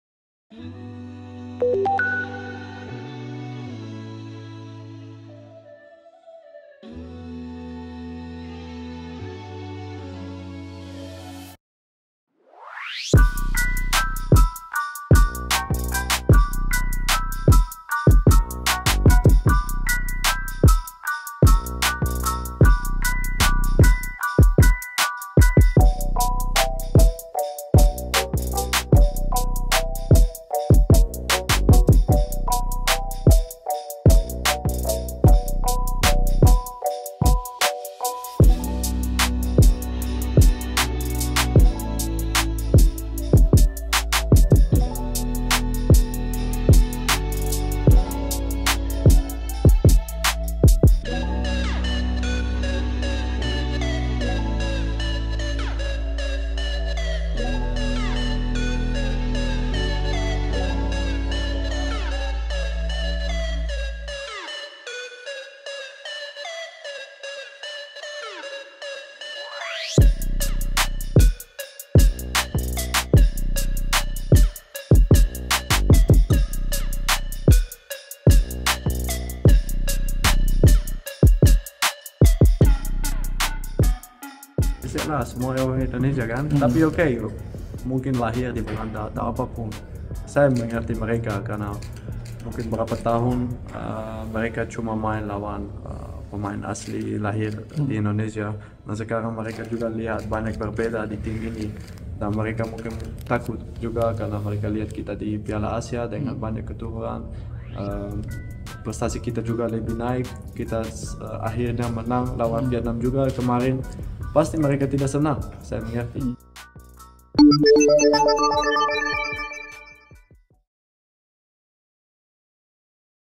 Marc Klok pahami kecemasan Vietnam melihat skuad timnas Indonesia
ANTARA - Gelandang tim nasional Indonesia Marc Klok memahami kecemasan tim nasional Vietnam yang menyebut tim Indonesia saat ini lebih seperti timnas Belanda. Menurut pemain Persib Bandung itu, tim Vietnam melihat perkembangan positif Tim Garuda saat Piala Asia 2023. (A Rauf Andar Adipati/Satrio Giri Marwanto/Farah Khadija)